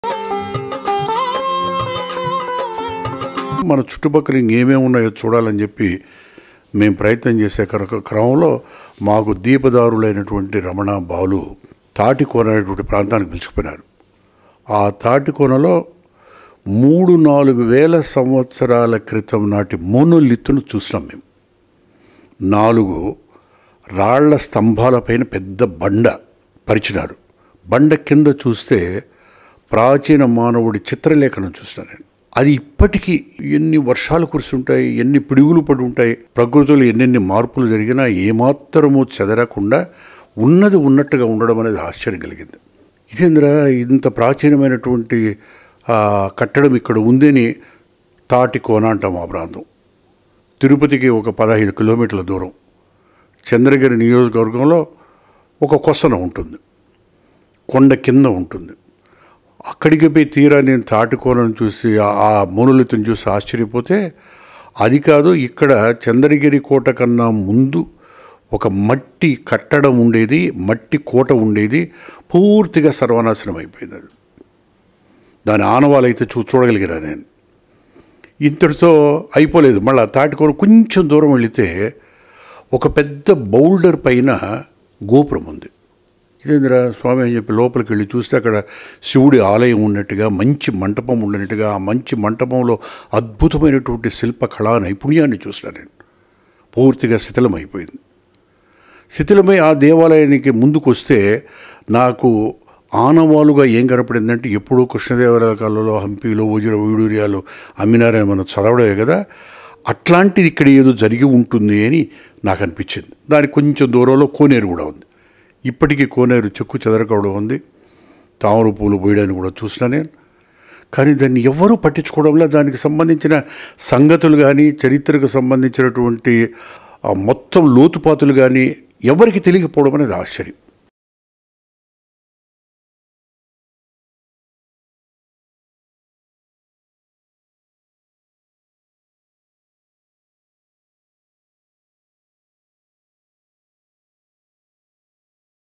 తిరుమల-తిరుపతి కొండల్లో వేల సంవత్సరాల మోనోలిత్ పెయింటింగ్స్.భూమన్ తిరుమలకొండపరిశోధకుడు| 𝐚𝐚𝐤𝐞𝐫𝐮 𝐍𝐄𝐖𝐒
తిరుమల-తిరుపతి కొండల్లో వేల సంవత్సరాల మోనోలిత్ పెయింటింగ్స్.భూమన్ తిరుమలకొండపరిశోధకుడు| 𝐚𝐚𝐤𝐞𝐫𝐮 𝐍𝐄𝐖𝐒 𝐓𝐞𝐥𝐮𝐠𝐮